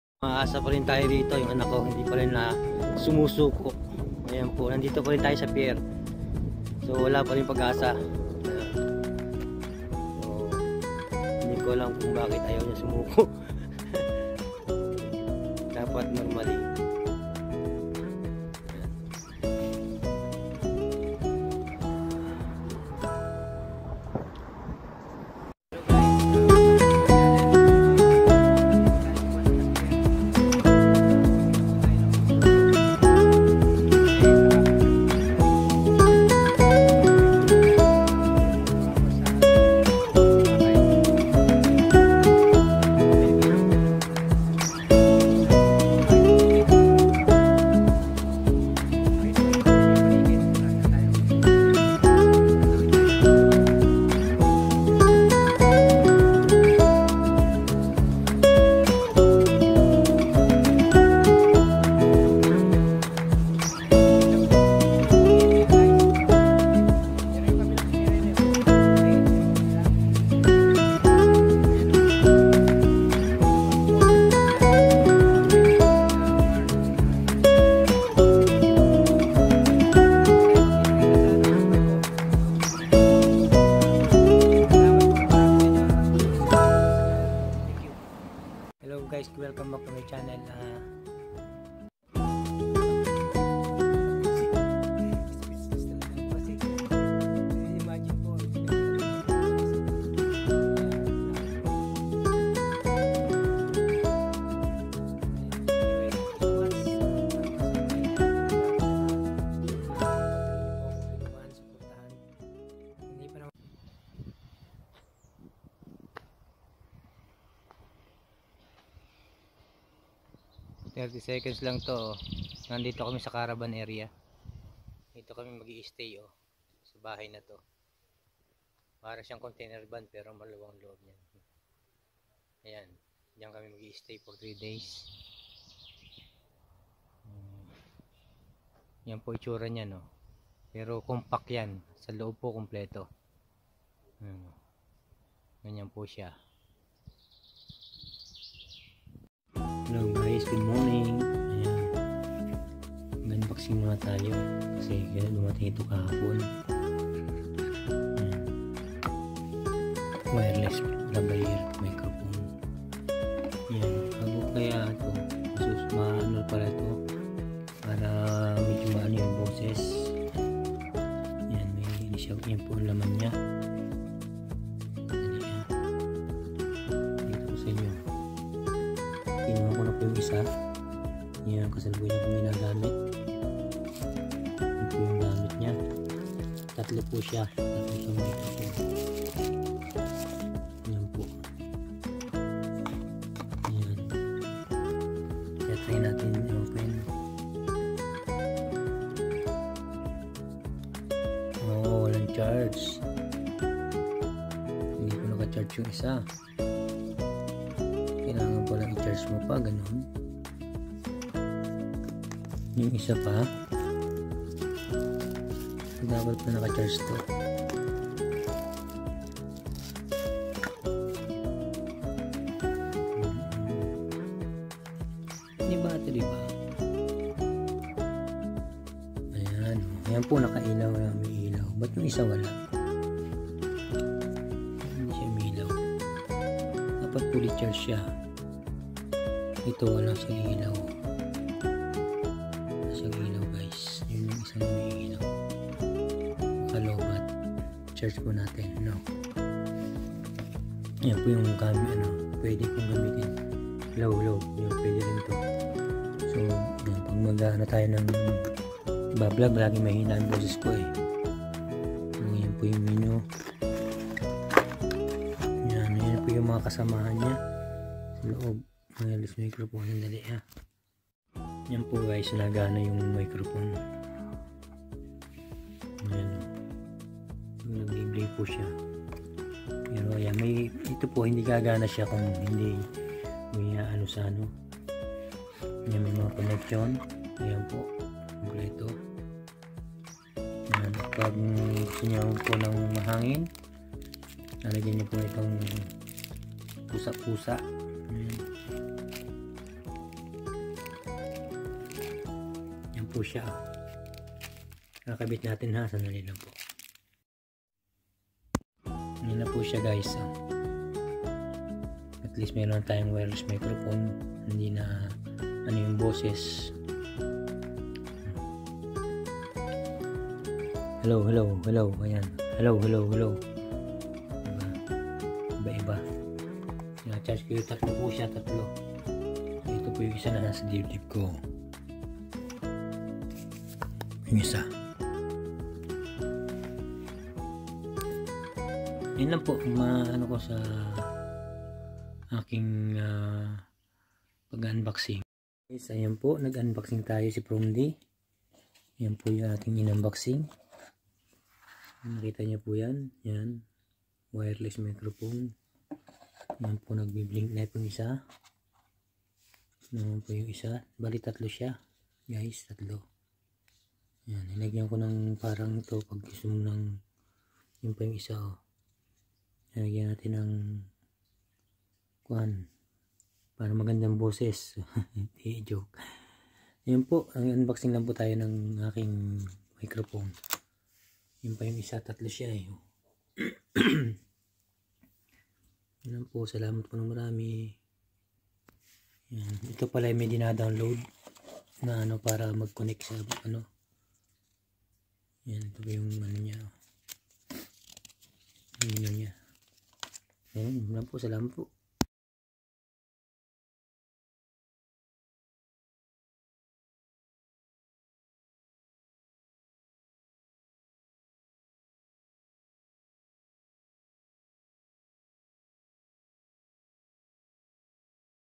UNBOXING MUNA TAYO!! GUMANA KAYA ITO?
MAHINA DAW BOSES KAYA SINUBUKAN KO ITO#WIRELESS#MICROPHONE#PROMDINGUK